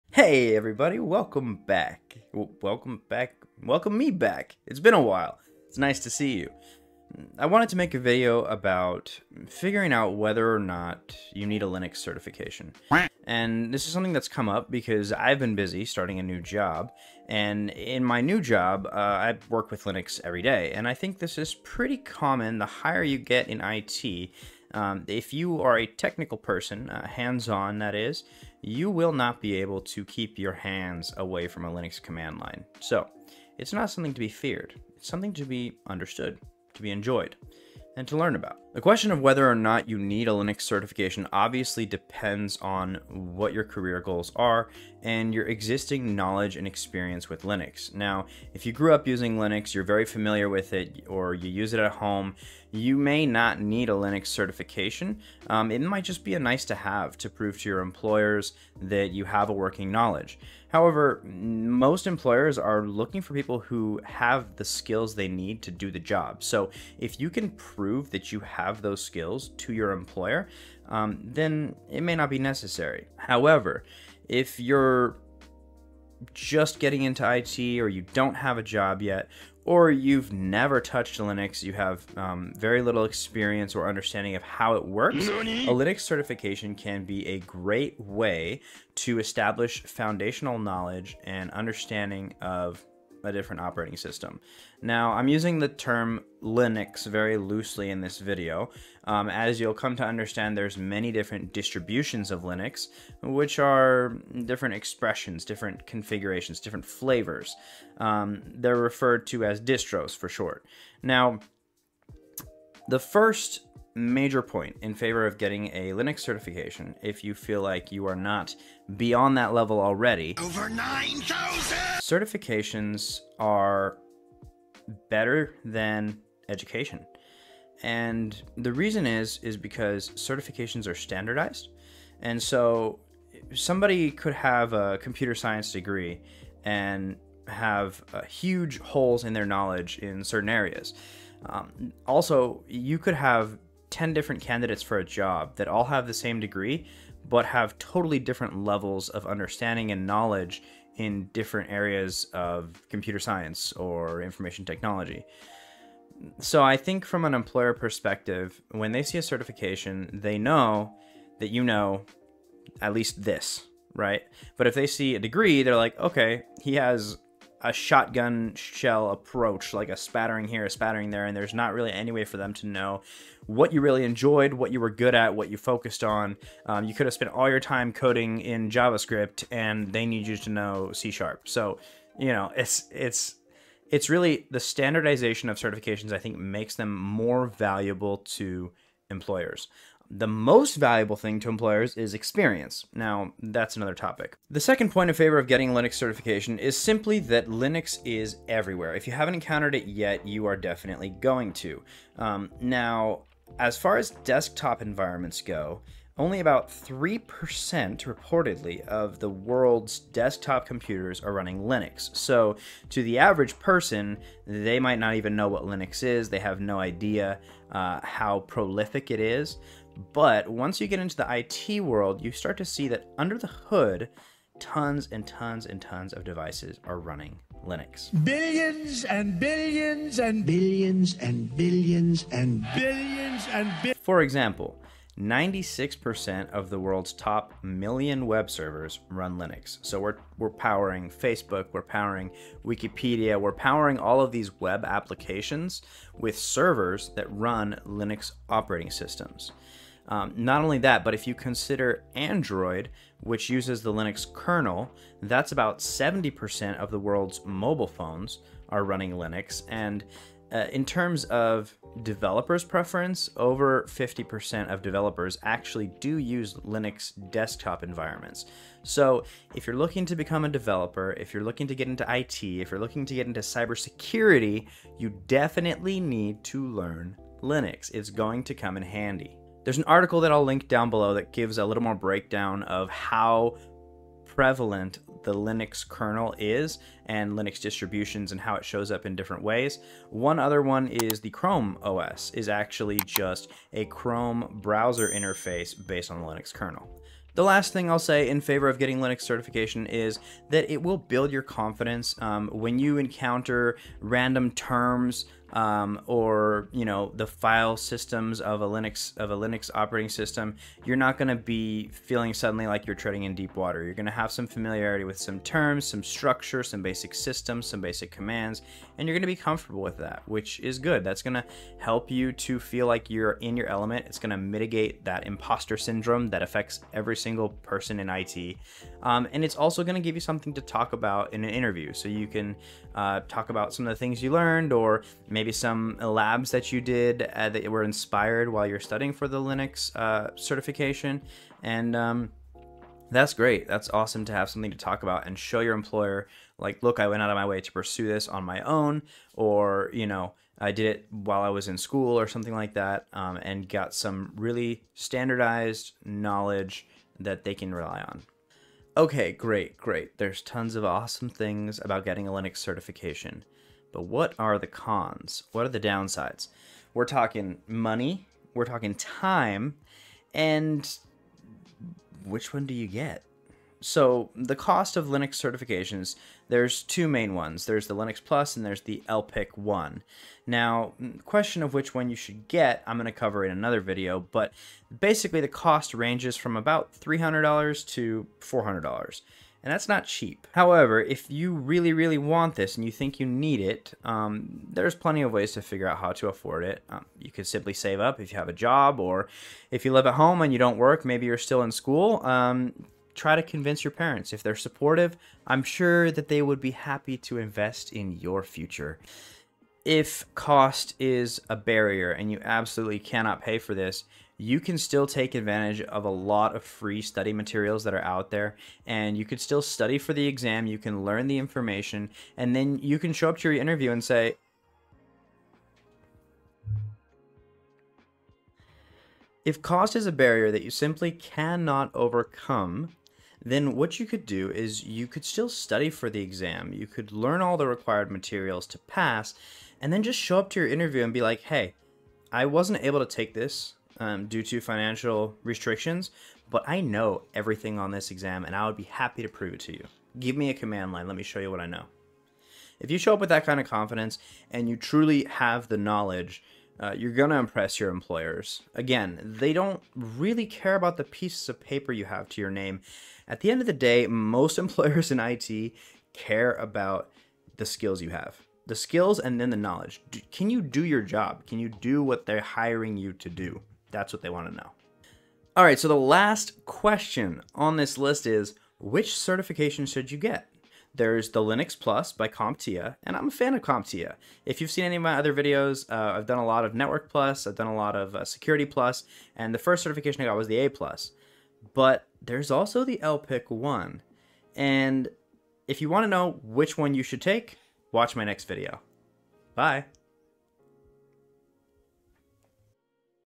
Do I Need a Linux Certification?
Do you need a Linux certification? Let's go over the pros and cons of getting one! Are there any alternatives?

Chapters
00:00 Intro
00:38 Do You Need It?
02:01 Pro #1
03:13 Pro #2
04:56 Pro #3
05:48 Con #1